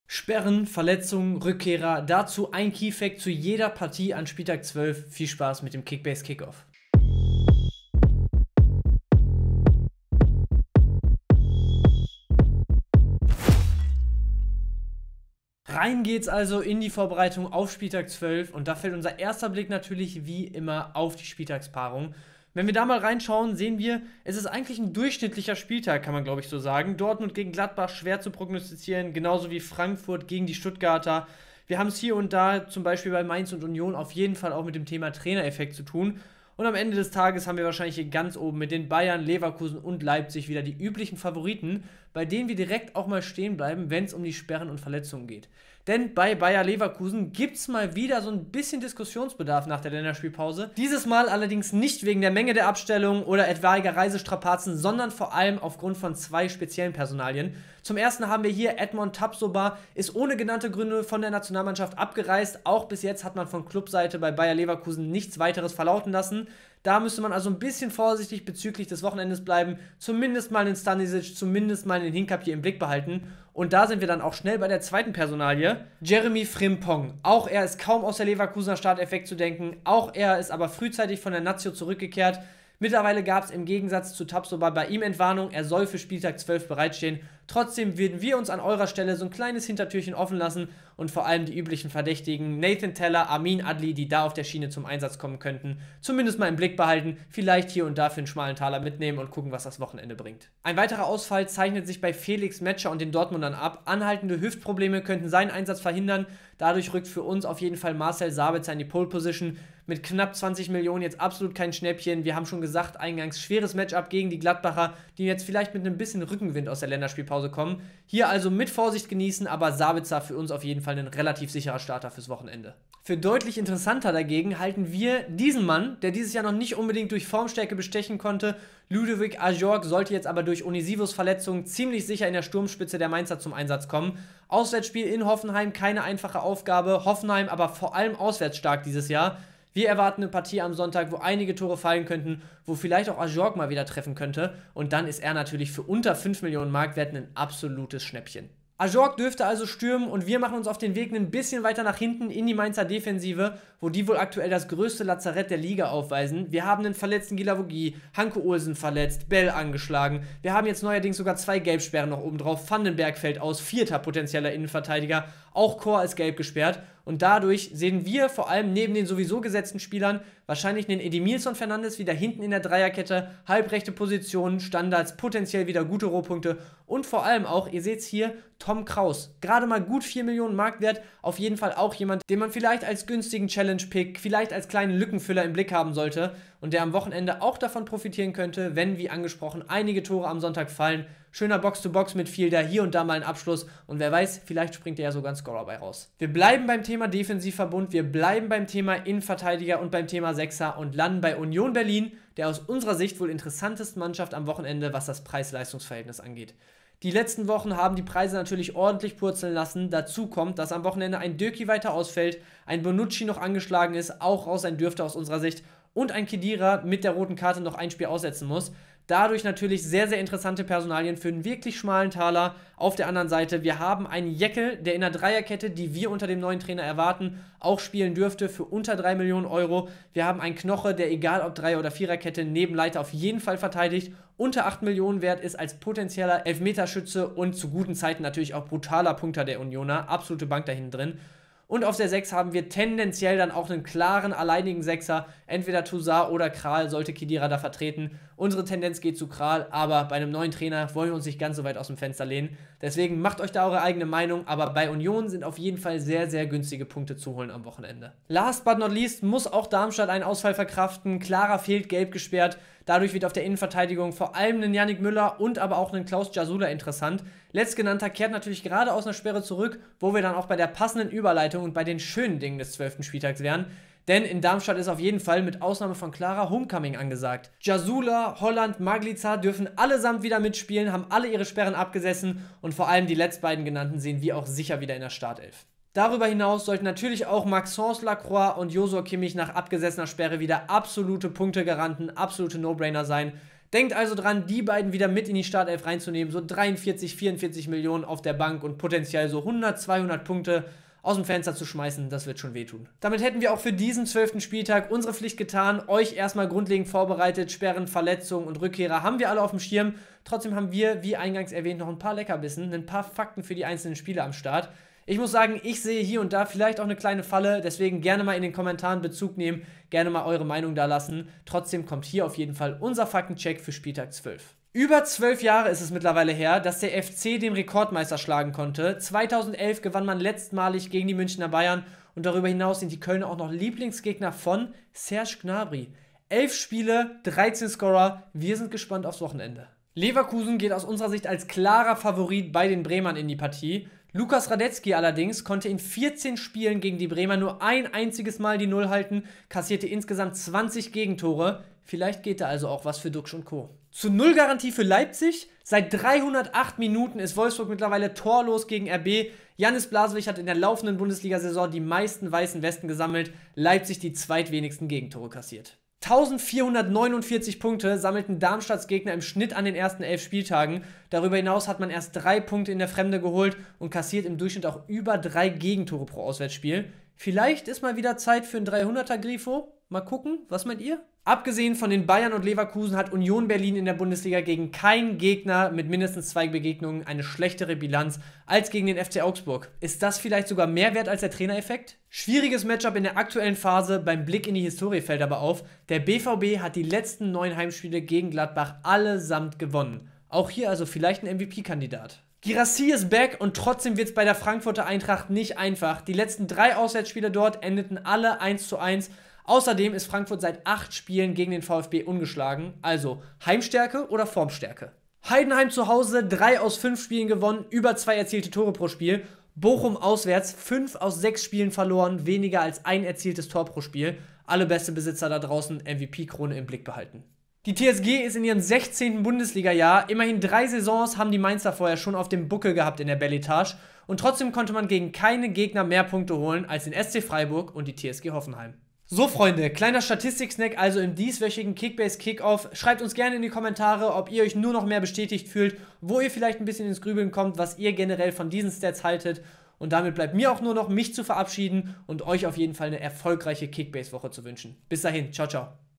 Statistiken oder Bauchgefühl?! | KICKBASE KICKOFF
Einige Favoriten, einige schwer zu prognostizierende Spiele - eigentlich ein ganz normaler Spieltag. Wir haben euch neben den Sperren, Verletzungen und Rückkehrern aber zu jeder Partie einen Fakt mitgebracht, der eure Denkmurmel in Bewegung bringen könnte!